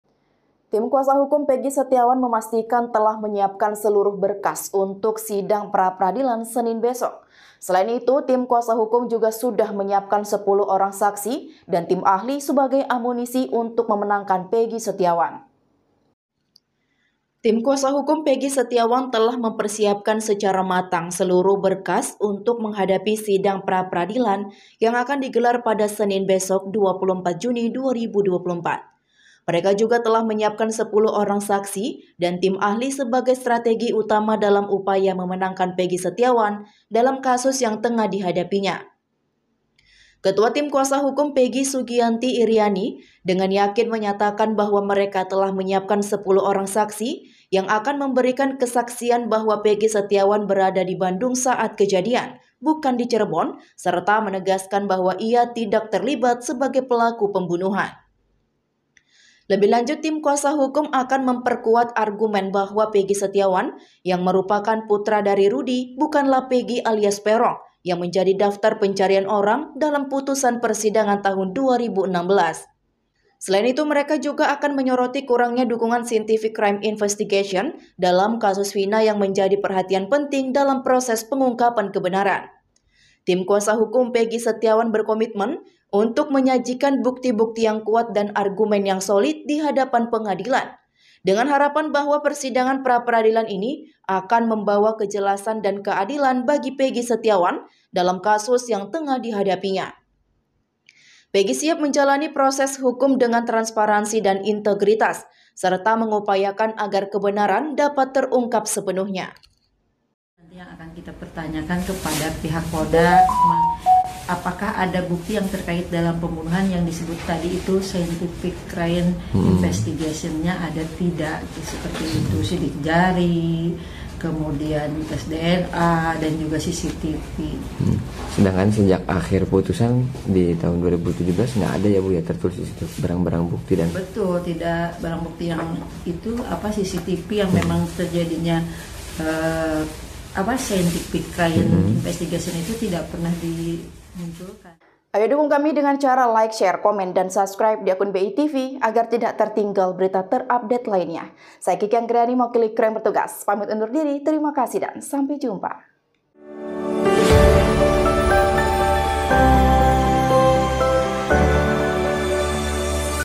Tim Kuasa Hukum Siapkan 10 Saksi dan Tim Ahli Untuk Memenangkan Pegi Setiawan
Tim Kuasa Hukum Pegi Setiawan memastikan telah menyiapkan seluruh berkas untuk Sidang Praperadilan, Senin besok (24/6).Selain itu, Tim Kuasa Hukum juga sudah menyiapkan 10 orang saksi dan Tim Ahli sebagai amunisi untuk memenangkan Pegi Setiawan.